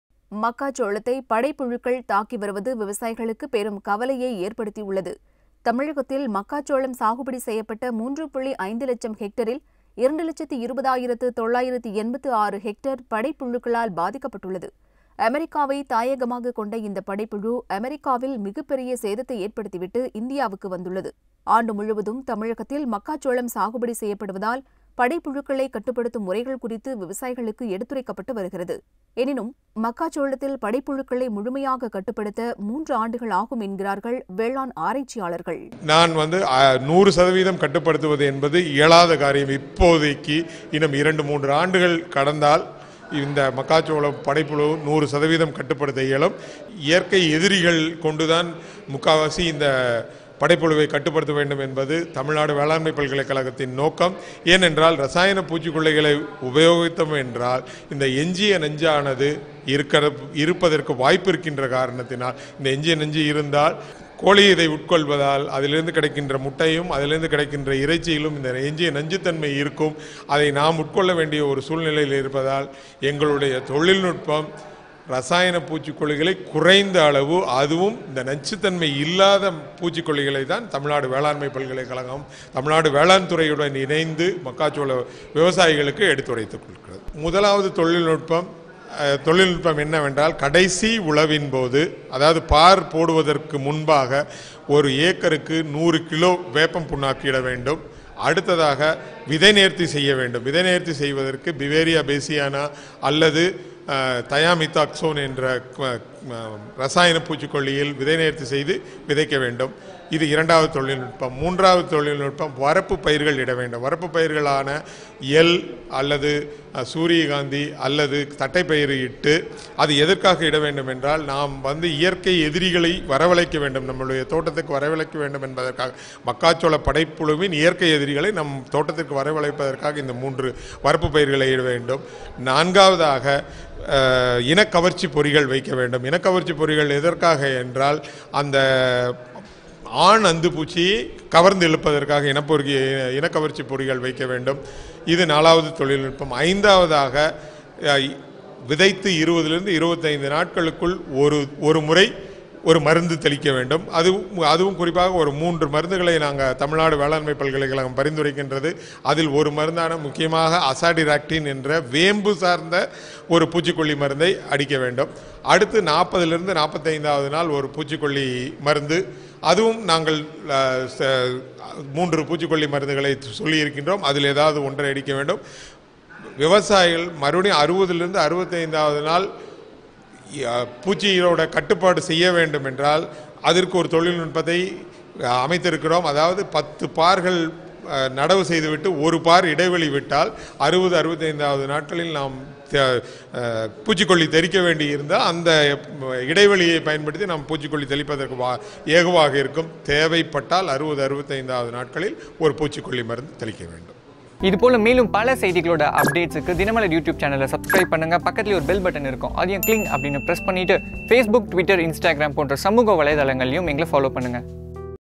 மக்காச்சோளத்தில் படைப்புழுக்களை கட்டுப்படுத்தும் முறைகள்
மக்காச்சோளத்தை படைப்புழுக்கள் தாக்கி வருவது
விவசாயிகளுக்கு பெரும் கவலையை ஏற்படுத்தியுள்ளது.
தமிழகத்தில் மக்காச்சோளம் சாகுபடி செய்யப்பட்ட
3.5 லட்சம் ஹெக்டரில் 2 லட்சத்து 20 ஆயிரத்து 986 ஹெக்டர்
படைப்புழுக்களால் பாதிக்கப்பட்டுள்ளது.
அமெரிக்காவை தாயகமாக கொண்ட இந்த படைப்புழு,
ஆப்பிரிக்காவில் மிகப்பெரிய சேதத்தை ஏற்படுத்தி விட்டு,
இந்தியாவிற்கு வந்துள்ளது. ஆண்டு முழுவதும் தமிழகத்தில்
மக்காச்சோளம் சாக்குபடி செய்யப்படுவதால், படைப்புழுக்களை
கட்டுப்படுத்தும் முறைகள் குறித்து விவசாயிகளுக்கு
எடுத்துரைக்கப்பட்டு வருகிறது.  எனினும் மக்காச்சோளத்தில்
படைப்புழுக்களை முழுமையாக கட்டுப்படுத்த